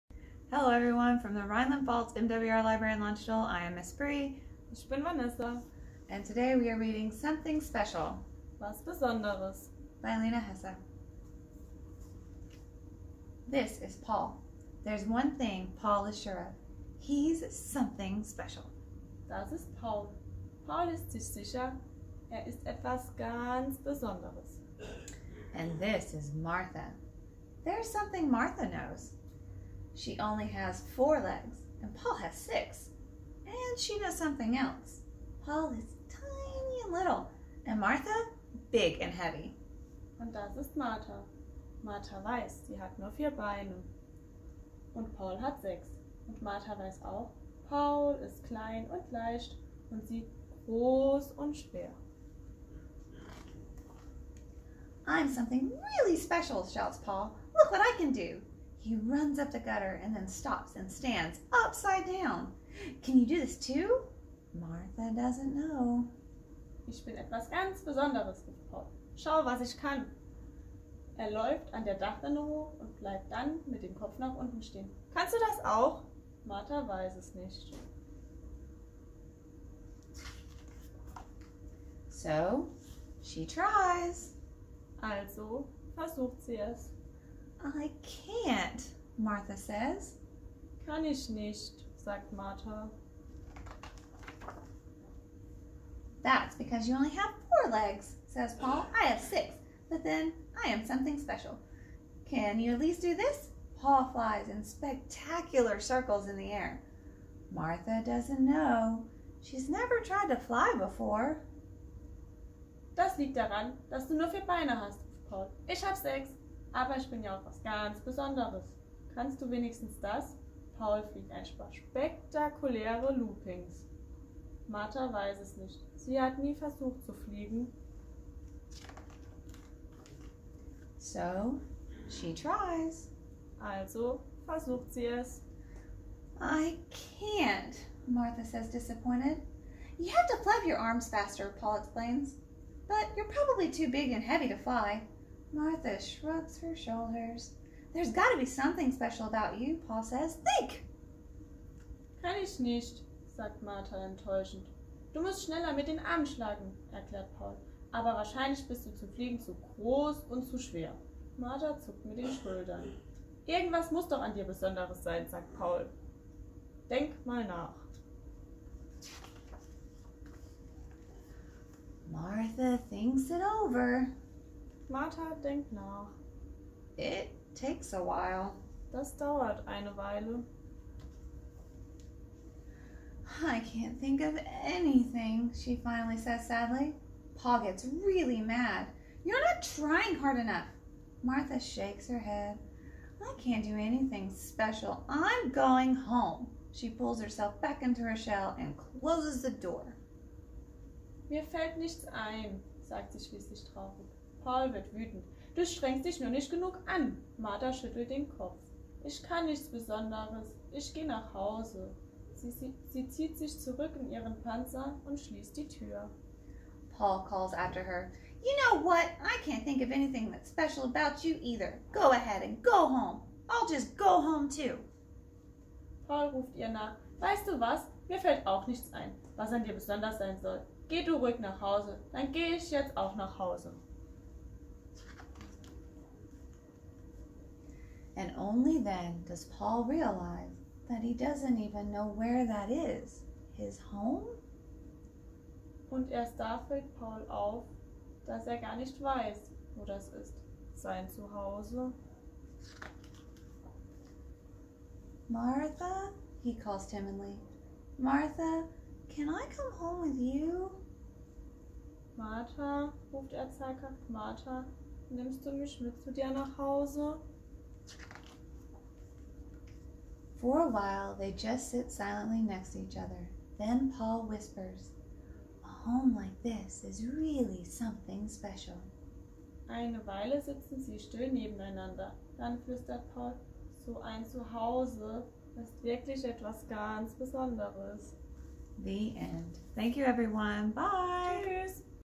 USAG RP Library Landstuhl- Storytime- Was Besonderes
Please watch as Mrs. Bri and Frau Vanessa bring you a fun and interactive German storytime.

Book: Was Besonderes

Author: Lena Hesse